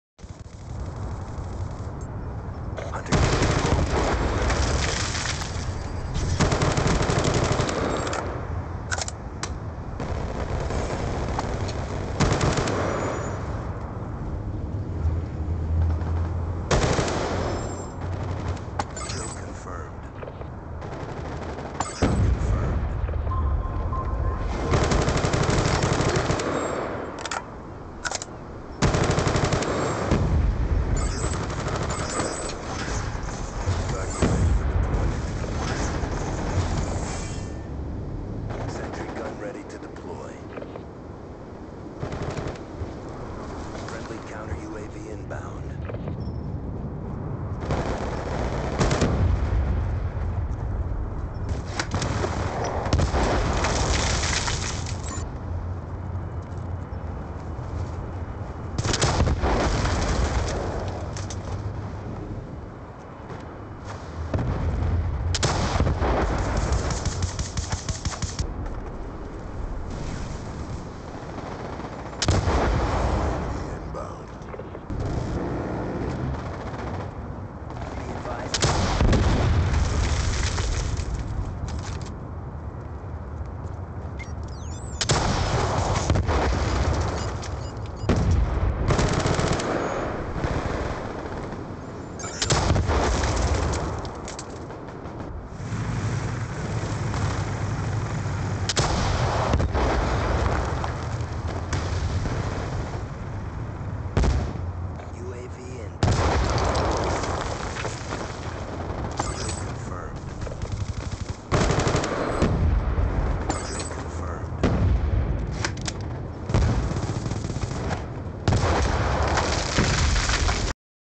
New Grind play
New Gun the Peacekeeper New stage grind. Check the game play out and tell me what you think.